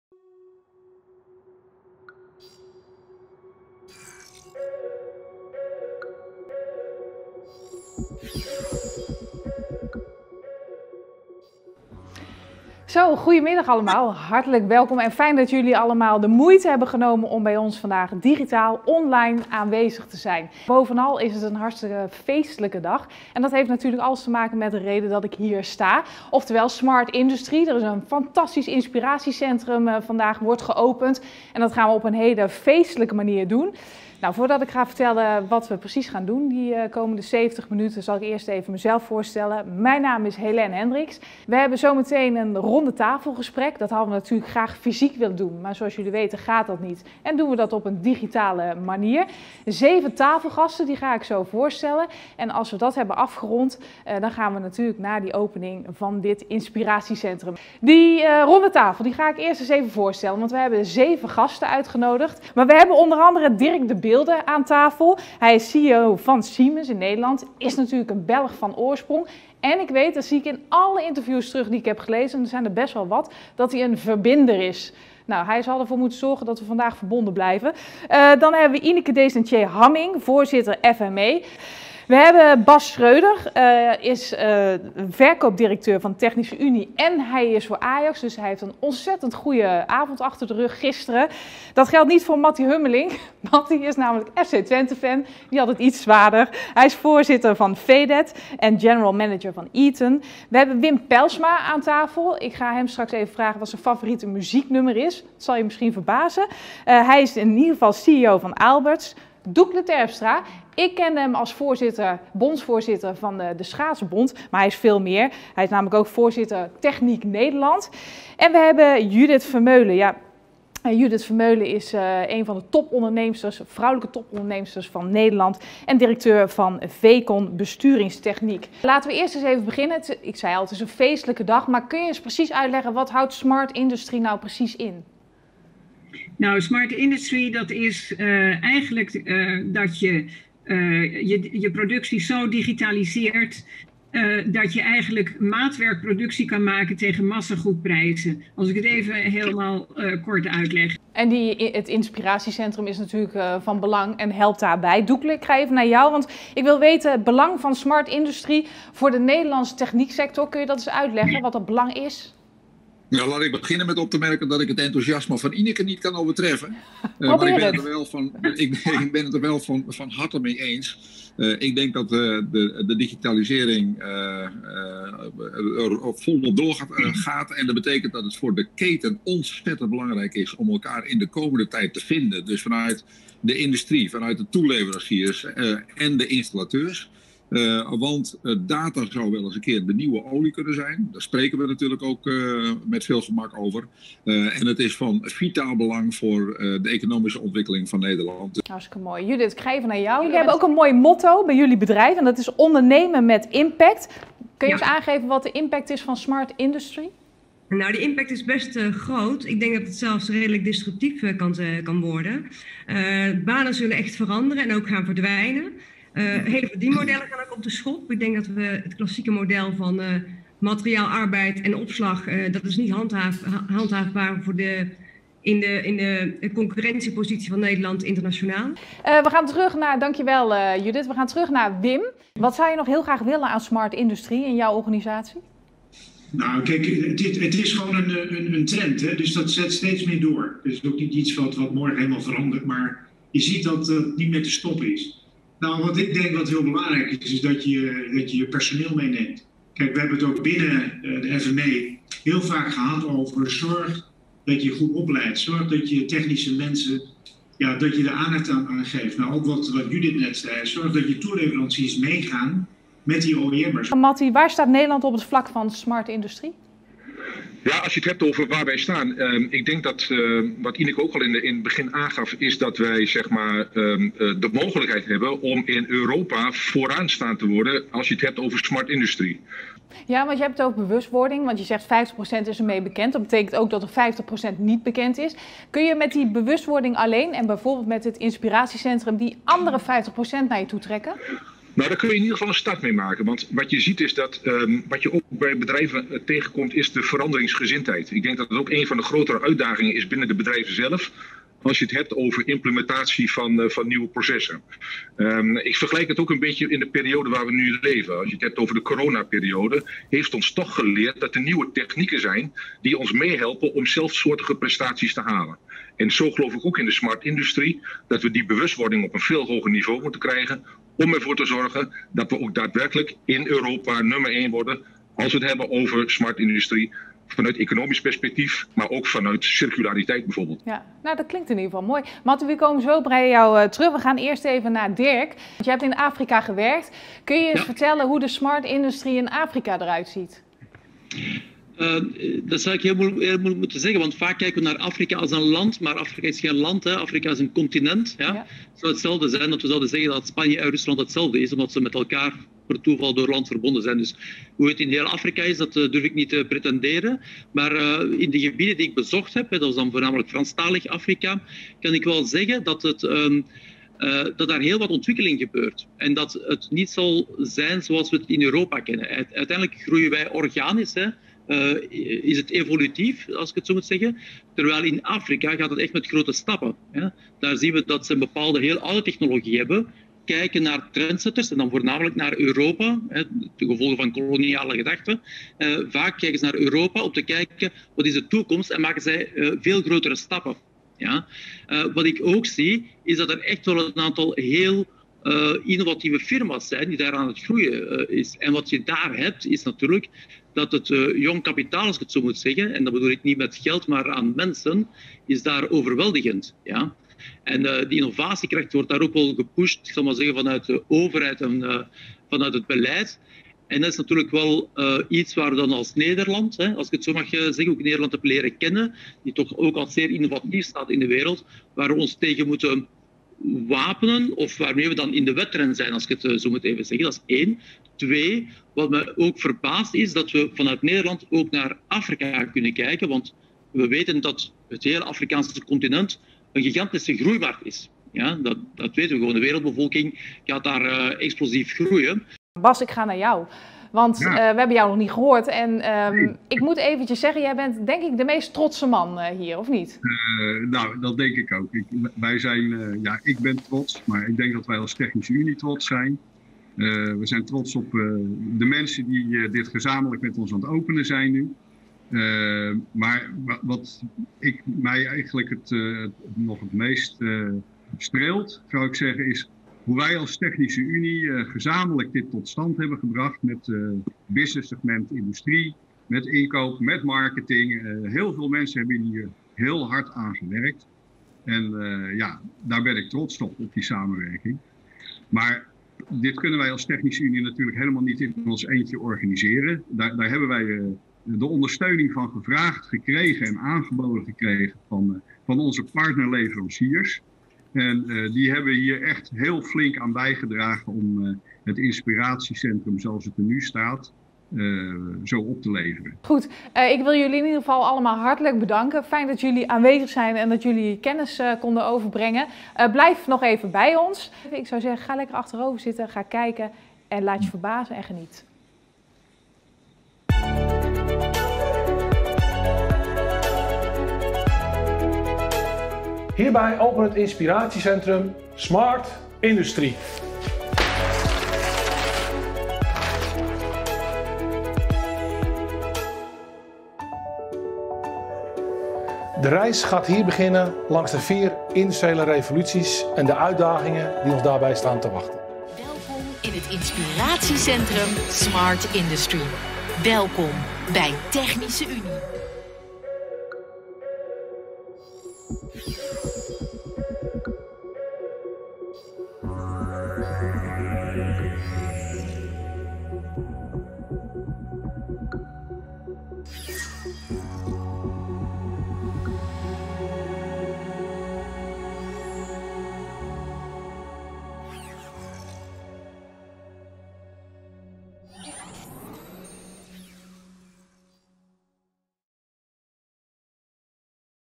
Opening Inspiratiecentrum Smart Industry | korte versie
Half januari 2021 werd in Eindhoven het Inspiratiecentrum Smart Industry geopend, met onder andere een roundtable met captains of industry.